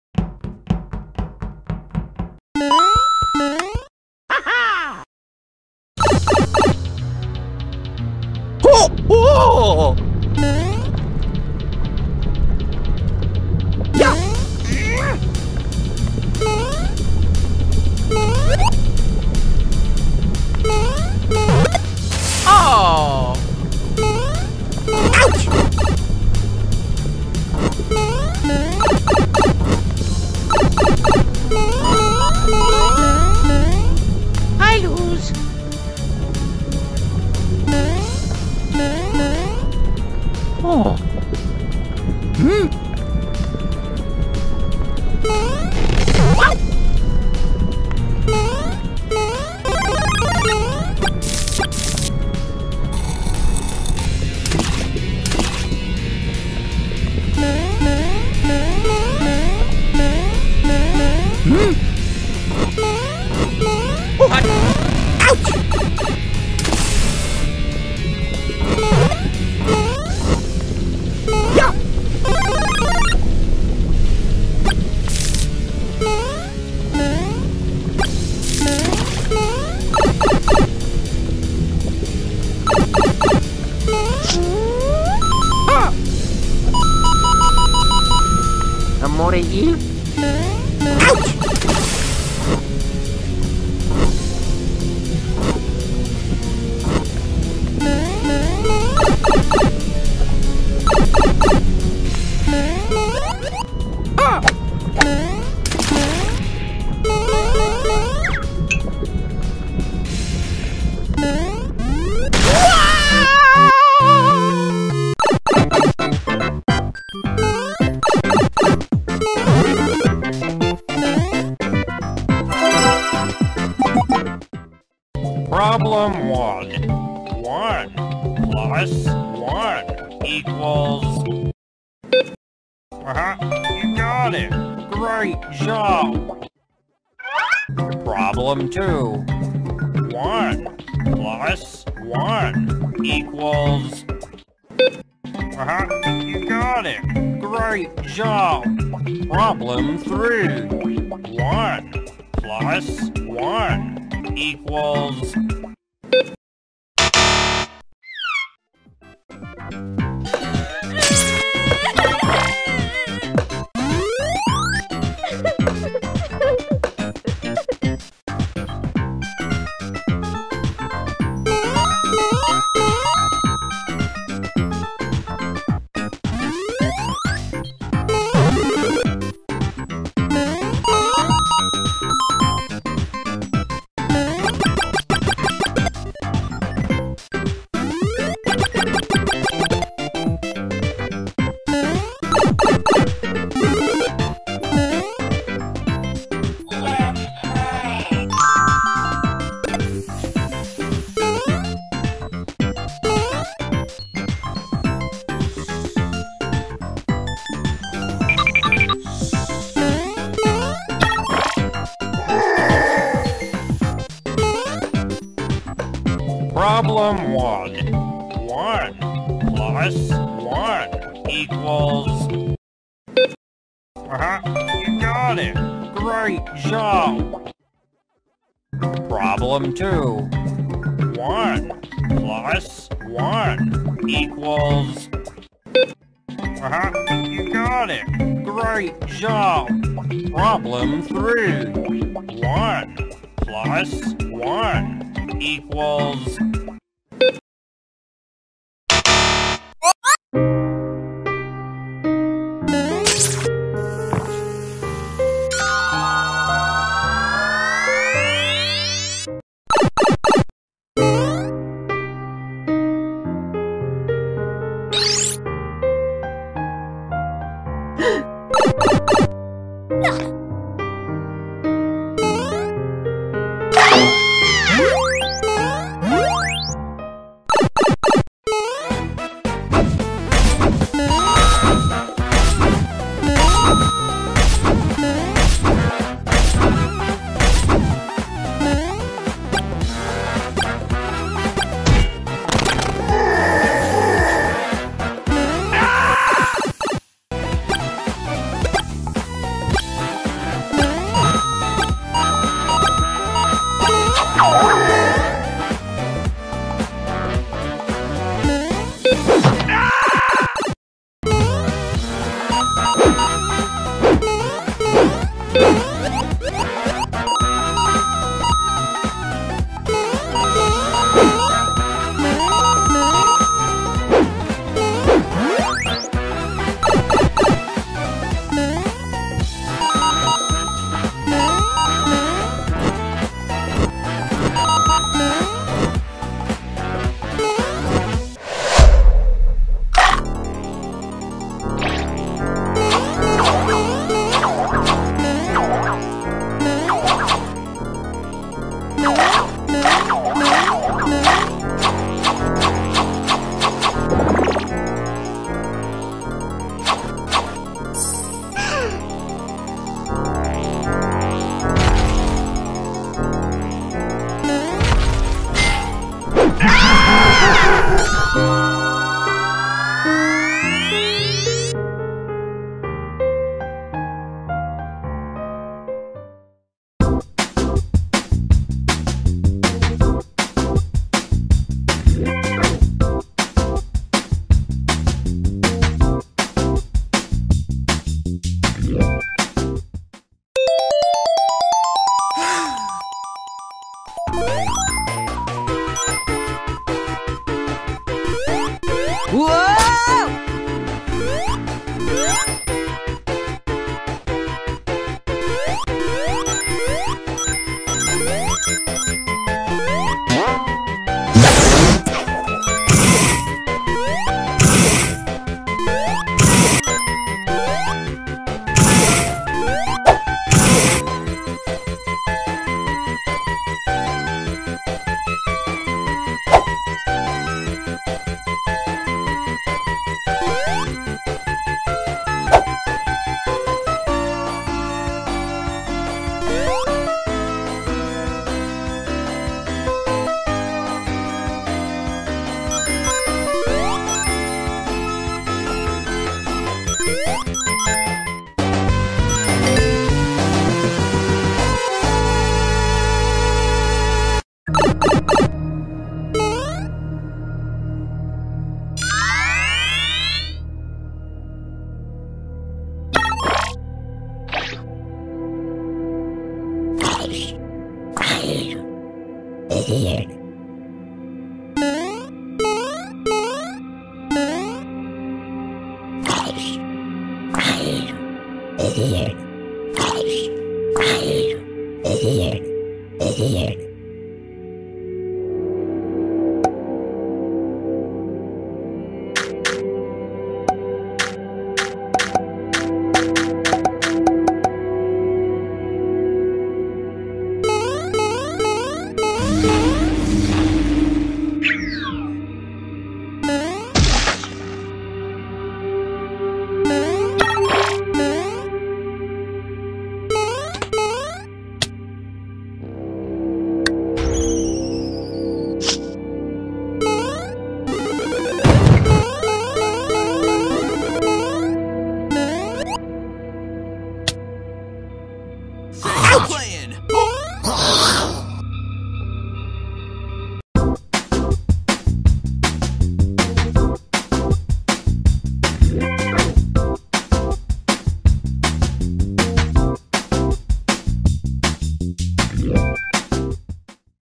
Pixnail: Mario and Hot vs Cold Challenge With Maze Mayhem - Super Mario FANTASTIC 4 | Pacman Parody
00:00 Mario and Hot vs Cold Challenge With Maze Mayhem
1:55 oh no! Mommy, Please come back home! Sonic don't cry
4:20 Sonic LOVE Amy ( Sonic + Amy =?)
6:31 Sonic's Ring Calamity
7:35 Jail Challenge for Sonic and Mario

All audio and visual content in this channel is owned by JVE CO. LTD